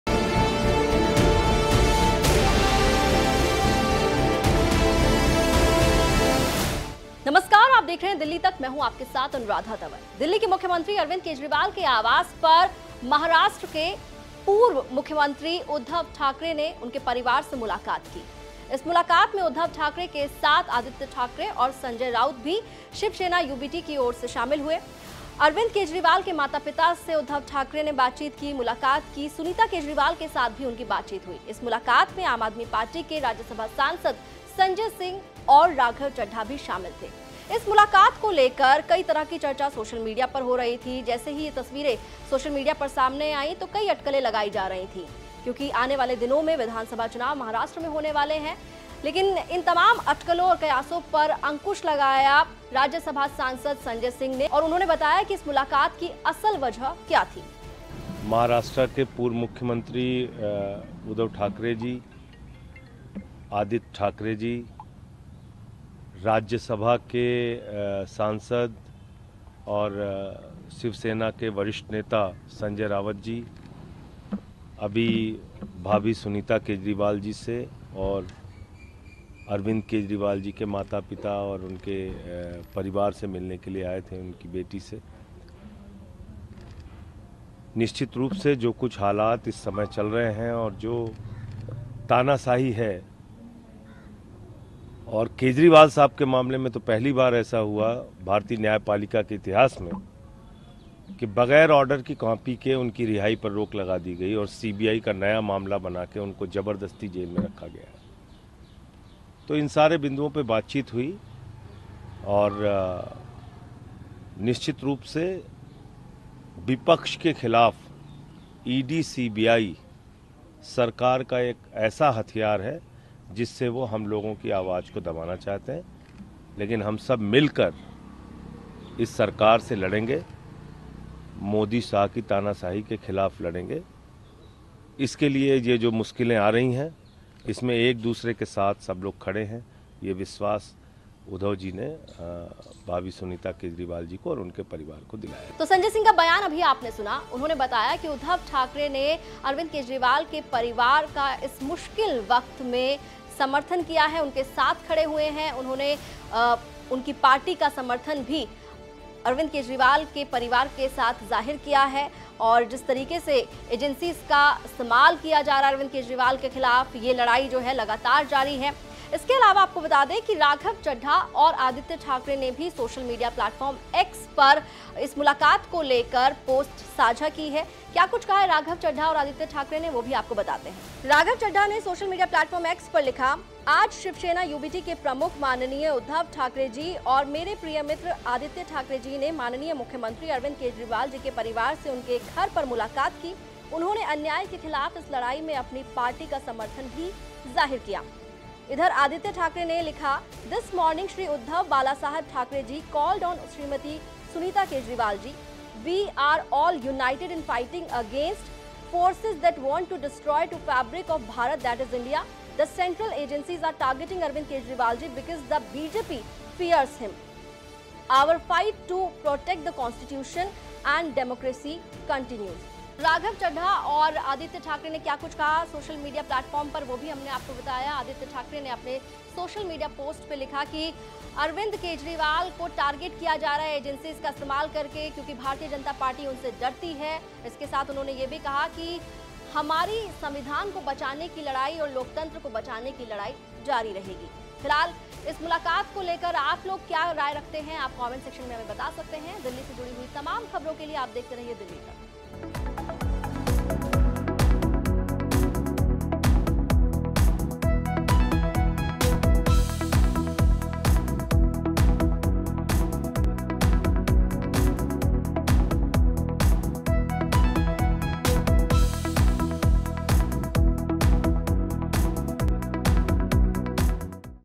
AAP AND SHIVSENA UBT : SUNITA KEJRIWAL के साथ UDHAV THACKERAY की बड़ी बैठक, हुआ ये अहम फैसला!
AAP AND SHIVSENA UBT : दिल्ली के मुख्यमंत्री अरविंद केजरीवाल के आवास पर SUNITA KEJRIWAL के साथ महाराष्ट्र के पूर्व मुख्यमंत्री और शिवसेना यूबीटी के प्रमुख UDHAV THACKERAY ने मुलाकात की। उद्धव ठाकरे की मुलाकार केजरीवाल के माता पिता से भी हुई। इस बड़ी बैठक में क्या हुआ ये बताया संजय सिंह ने।

Follow us at:

Twitter Download Tak App: tak.live/dilli-tak